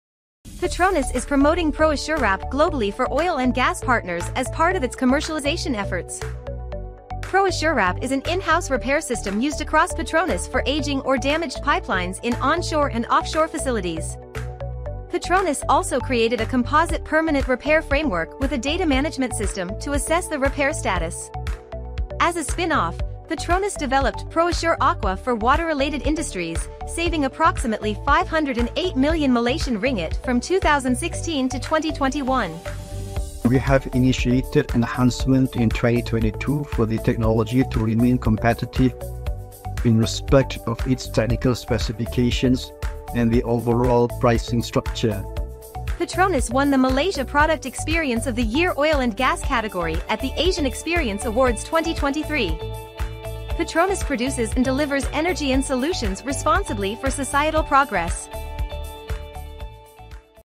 Asian Experience Awards 2023 Winner: Petroliam Nasional Berhad ProAssure Team
Revolutionising pipeline care, PETRONAS introduces ProAssure-Wrap globally, a cutting-edge repair system for ageing pipelines. This award-winning solution celebrates excellence in oil and gas innovation.